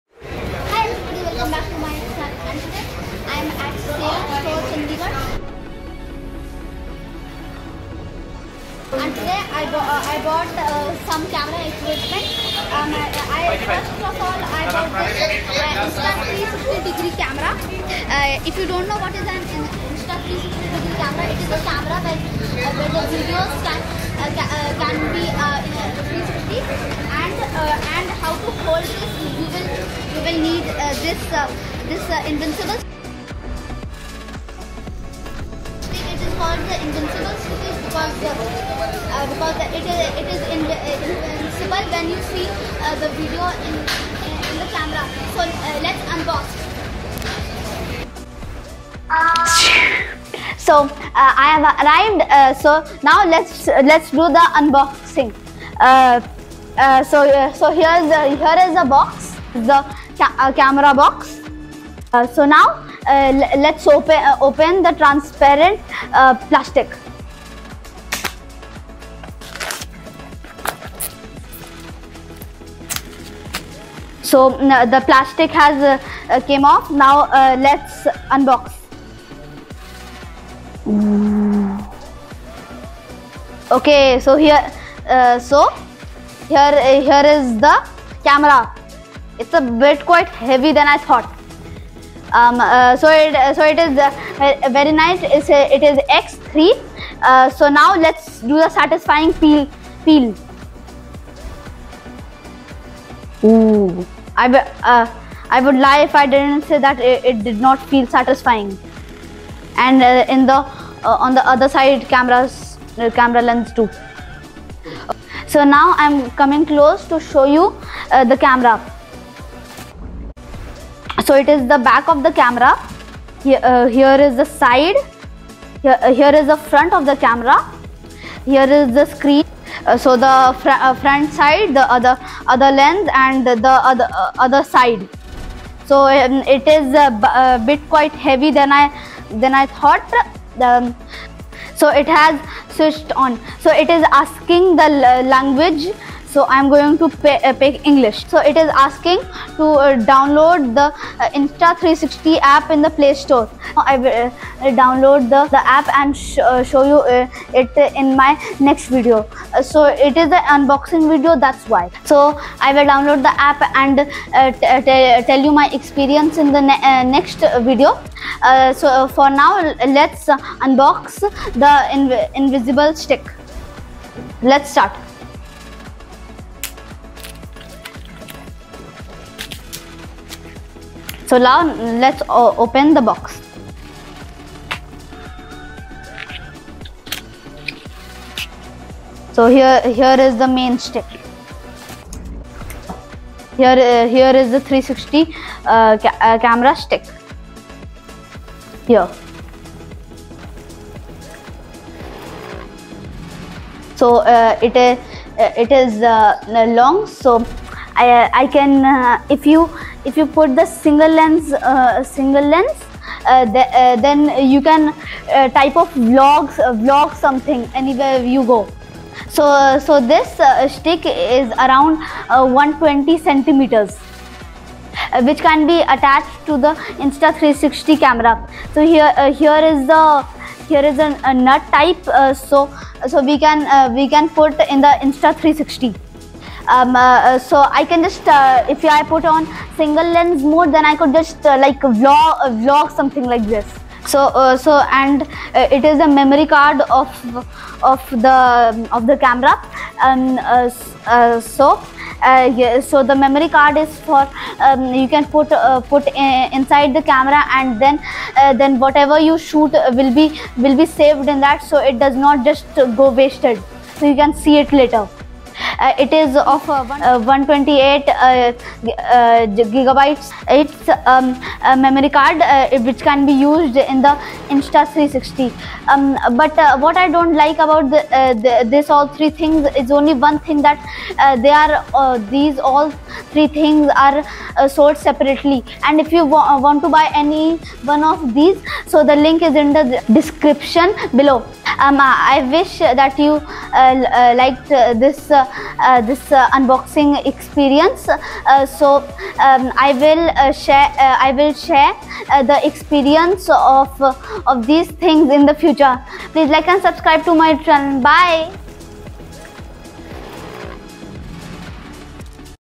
Atharva unboxes Insta360 X3 camera, stick and a memory card !! He is excited to create new videos !!
* Noise till 47 seconds !!

Video Description -

Atharav likes to create craft, art and make videos. He gets a new camera Insta360 X3 to make 360 degree videos. He is excited to explore the new possibilities of content creation.

Amazon link of this product